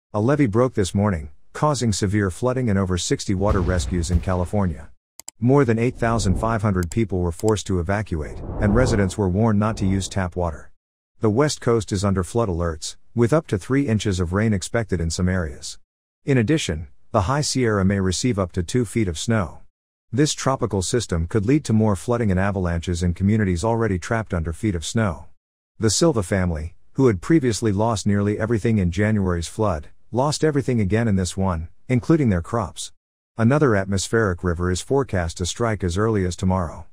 California Hit by Devastating Floods and Levee Breaks, Thousands Forced to Evacuate
California has been hit by severe floods and levee breaks, causing widespread damage and forcing more than 8,500 people to evacuate. Over 60 water rescues have been conducted, and residents have been warned not to use tap water. The West Coast remains under flood alerts, with up to three inches of rain expected in some areas. The situation is further complicated by the possibility of heavy snowfall, which could lead to more flooding and avalanches in communities already struggling with feet of snow. Another atmospheric river is forecast to strike soon, making the situation even more dire for those affected.